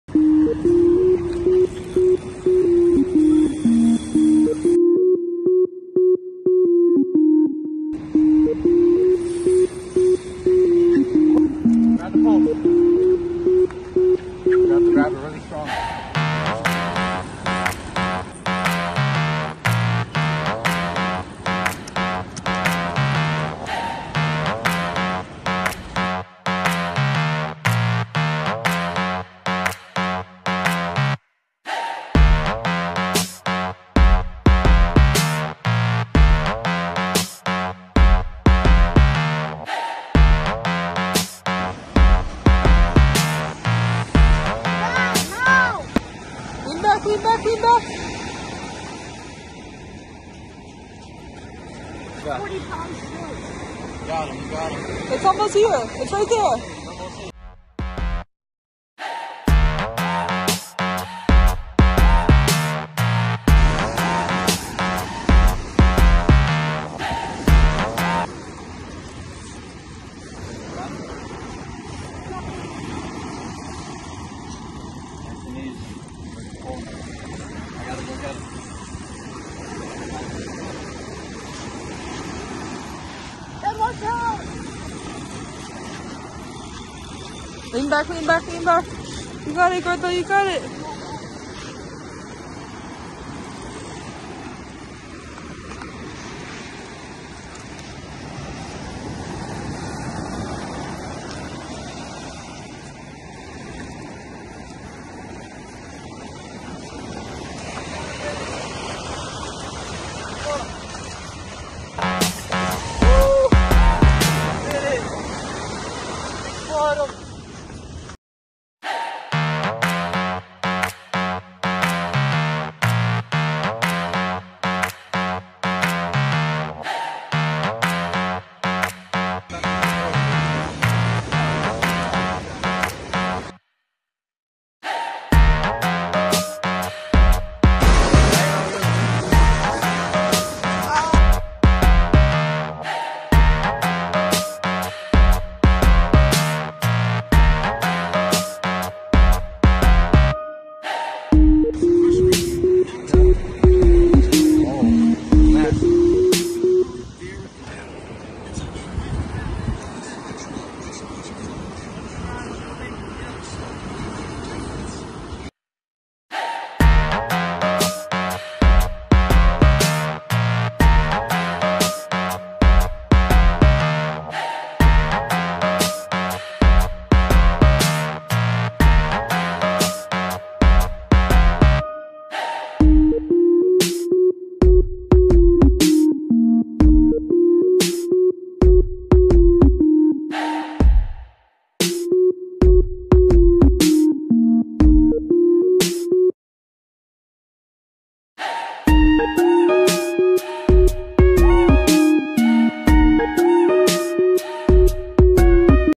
Father and son Shark Attack!?
My son landed his first shark

I don’t own copy rights to the music in this video ​⁠​⁠​⁠@VlogNoCopyrightMusic